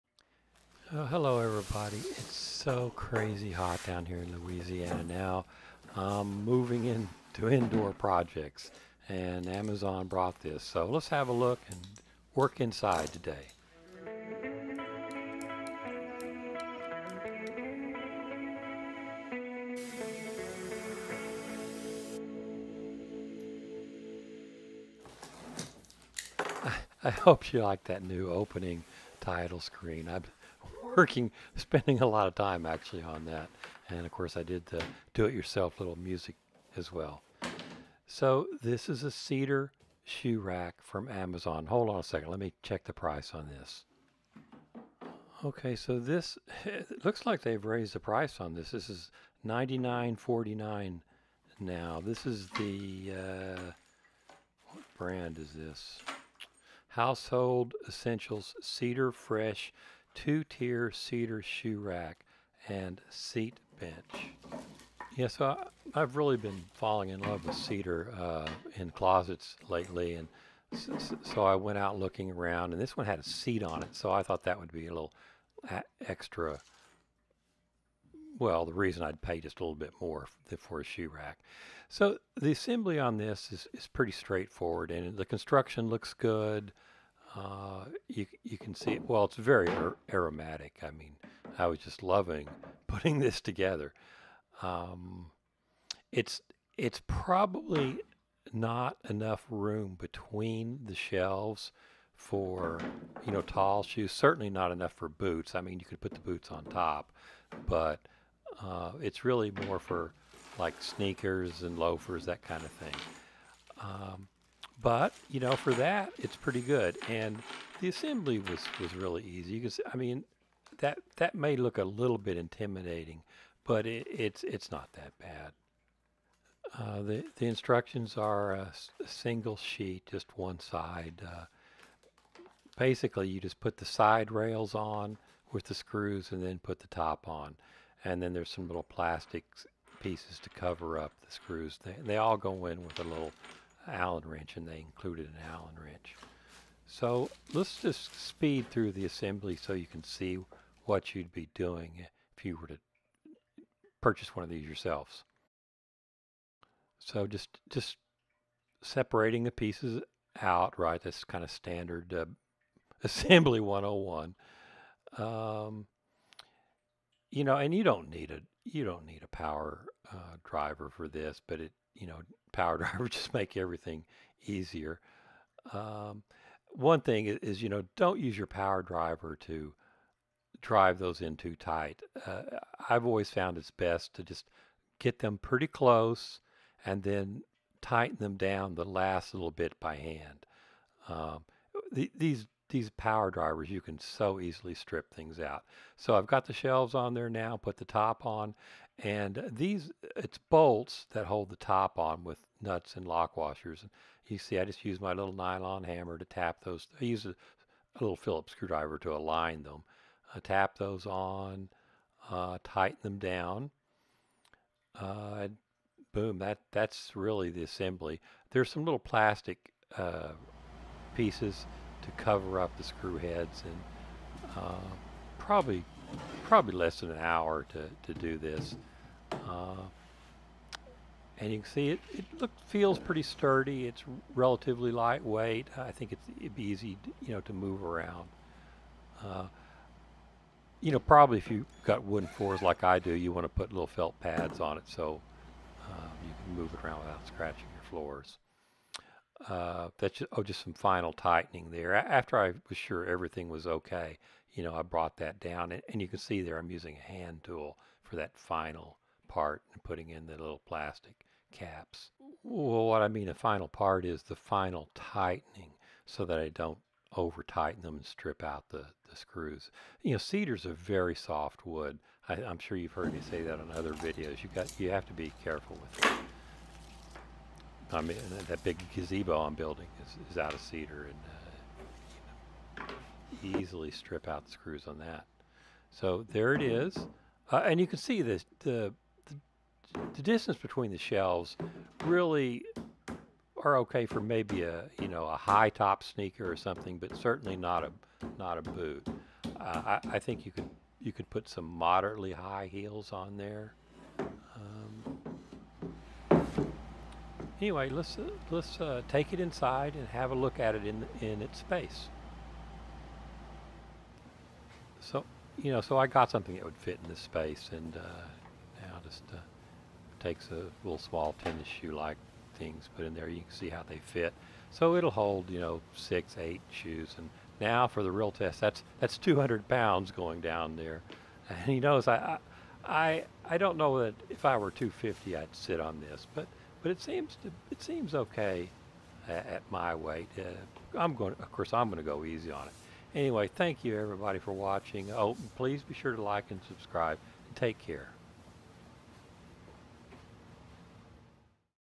Cedar shoe rack unbox and assembly
Cedar shoe rack unbox and assembly.  Household Essentials CedarFresh 2-tier Cedar shoe rack and seat bench $99.49.  Unbox, assemble and seat weight testing.  Easy DIY furniture assemble, you can assemble this.  Looks great, smells great.